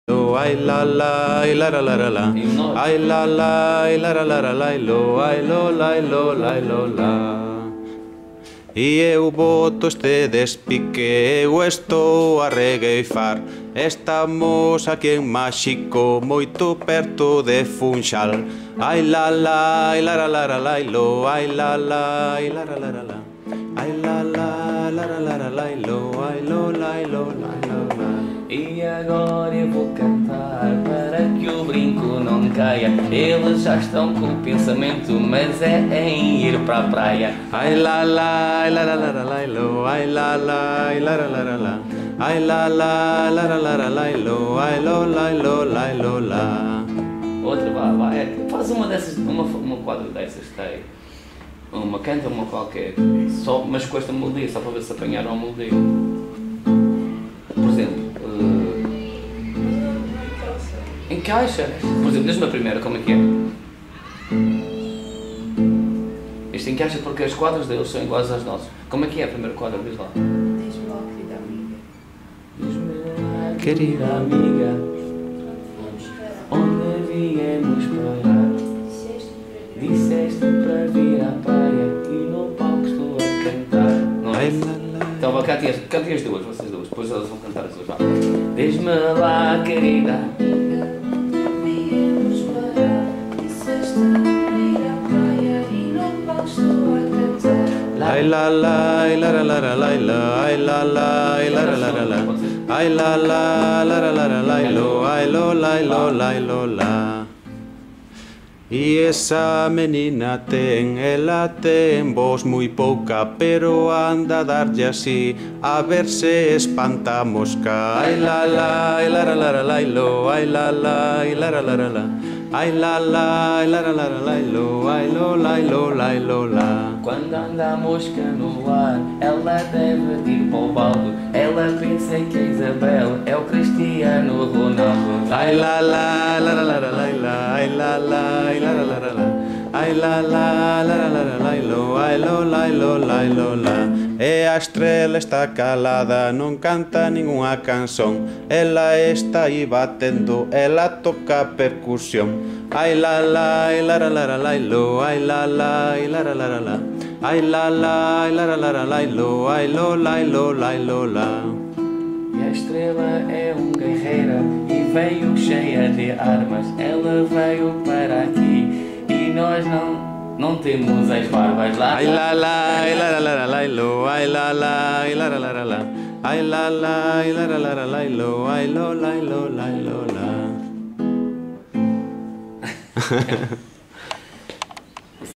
Aprendendo a melodia da regueifa na ilha da Madeira
Este é o grupo de cordas Machetes de Machim, de Machico, na Ilha da Madeira. Está começando a aprender improviso, com o professor Roberto Moniz. É uma nova geração de rapazes e rapazes que gostam da música e do canto improvisado. E aqui está a aprender como é a nossa regueifa.

Na Madeira também há vontade de impulsar o despique -mui parecido com a nossa regueifa- entre rapazes e rapazas. O improviso na Madeira leva acompanhamento musical, habitualmente cordofones, ou acordeom ou harmônica.